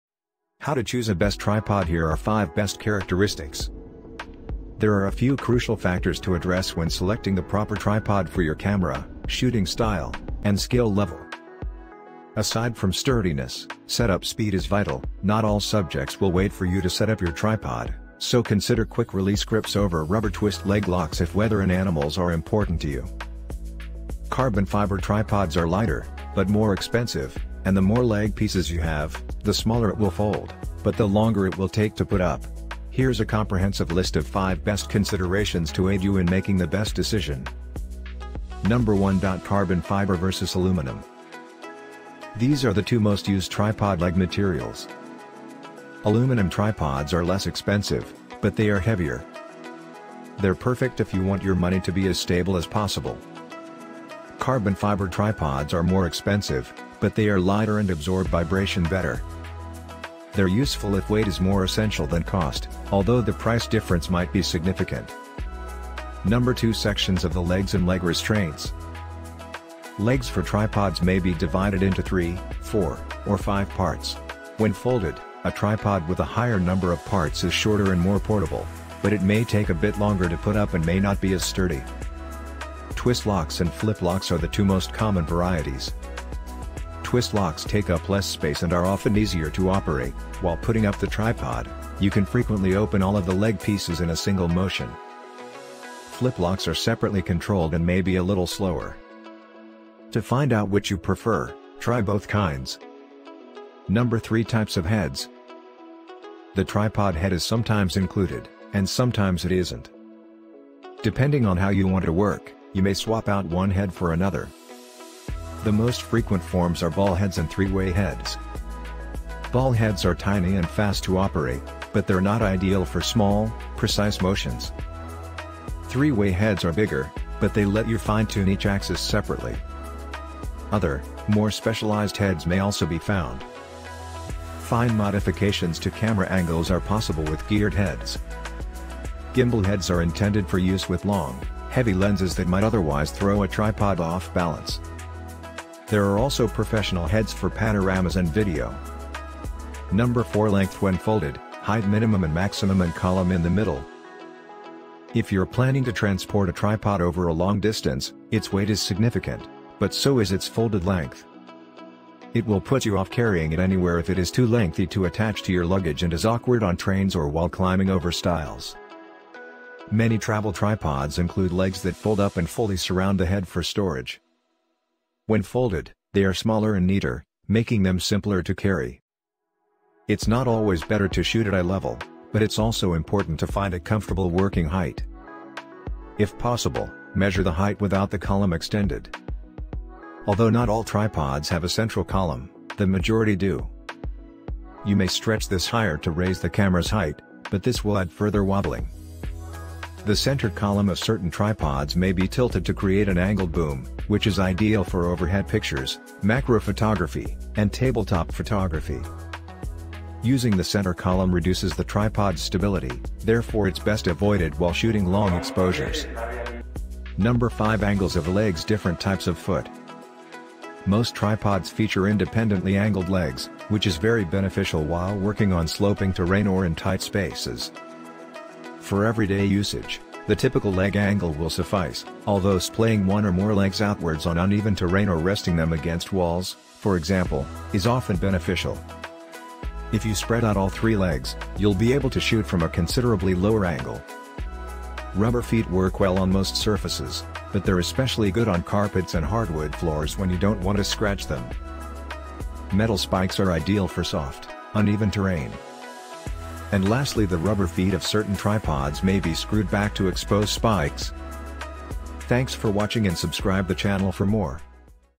Best Tripod, Here are 5 Best Characteristics to choose the Best.
The good tripods should undoubtedly be the first gear purchase by every photographer.
Having a sturdy, trustworthy tripod allows you to do a lot of different things with your camera that you wouldn't be able to do without it.
Here's our selection for the  5 best tripods for all budgets, along with expert advice on reasons  to choose and reasons to avoid  the ideal tripod for your photography style.